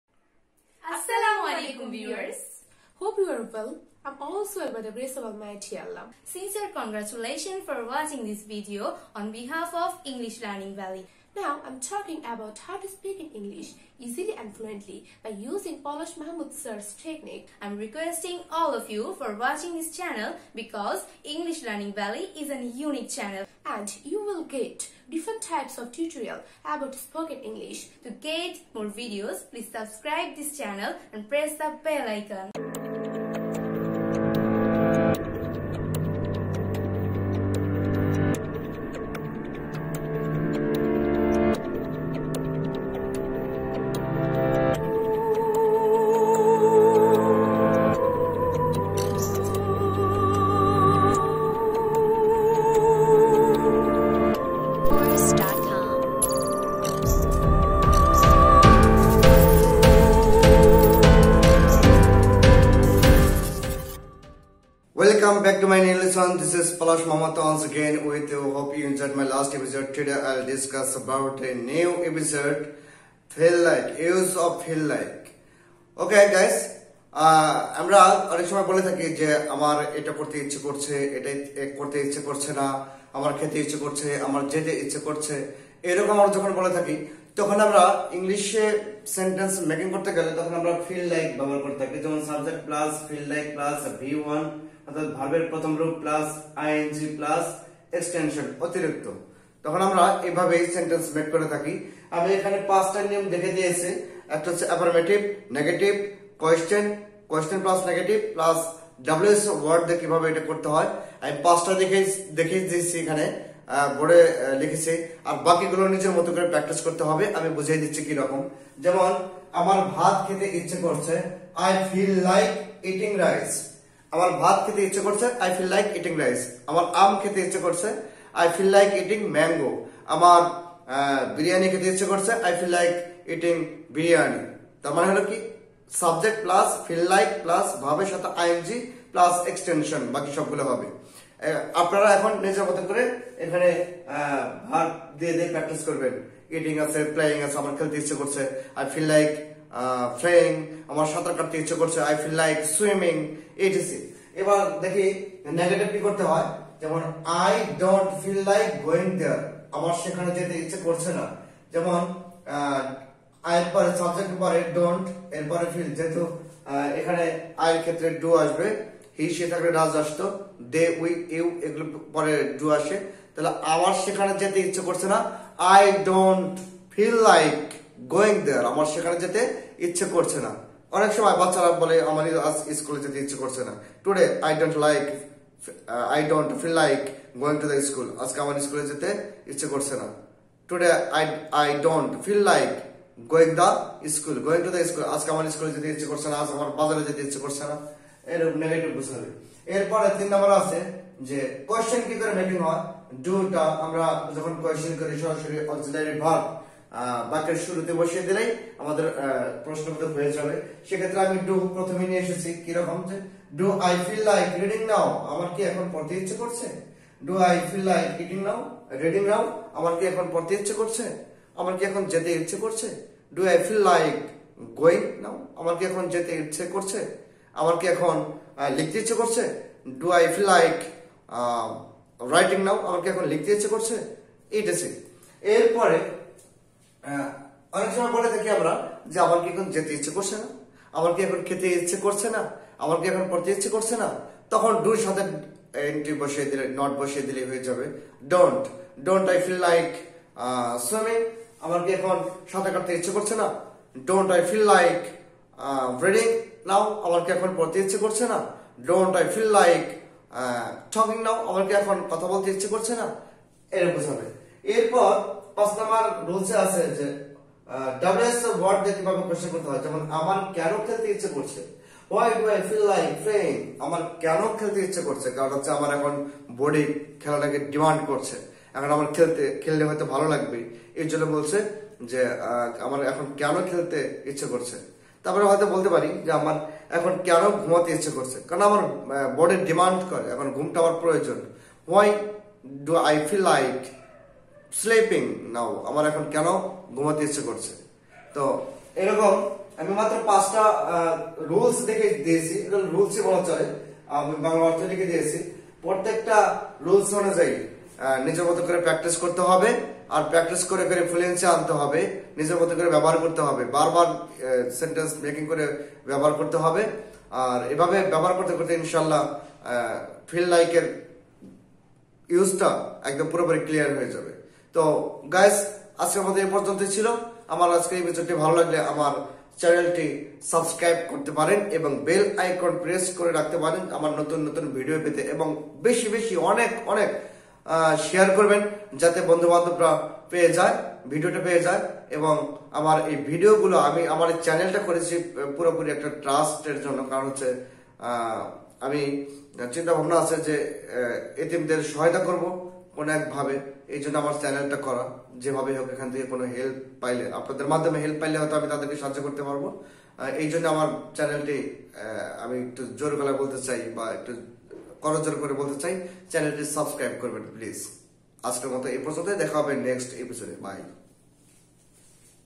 How to speak in English easily and fluently | Part 19
In This Video I Will Show You How to speak in English easily and fluently.

Channel : English Learning Valley
Pressenter : Palash Mahmud